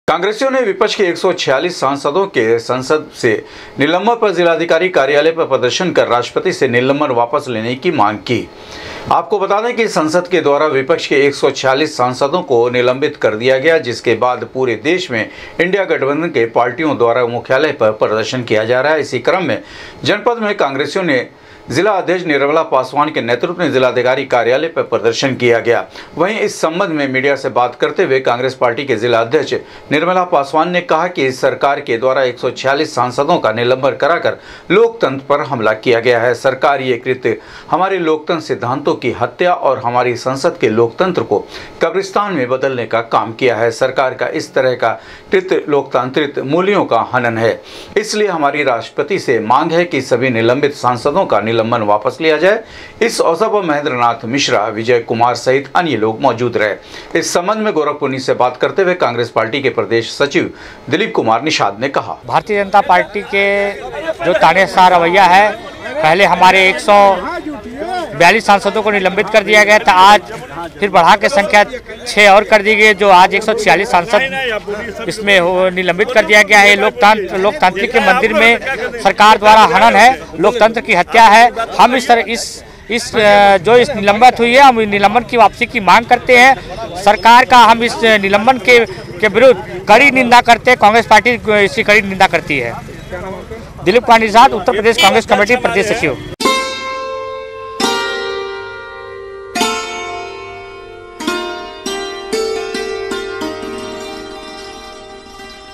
कांग्रेसियों ने विपक्ष के 146 संसादो को संसद से निलंबन पर किया प्रदर्शन
कांग्रेसियों ने विपक्ष के 146 संसादो को संसद से निलंबन पर जिलाध्यक्ष निर्मला पासवान के नेतृव में जिलाधिकारी कार्यलय पर किया प्रदर्शन,देश की राष्ट्रपति से सभी संसादो के निलंबन वापस लेने की,की मांग//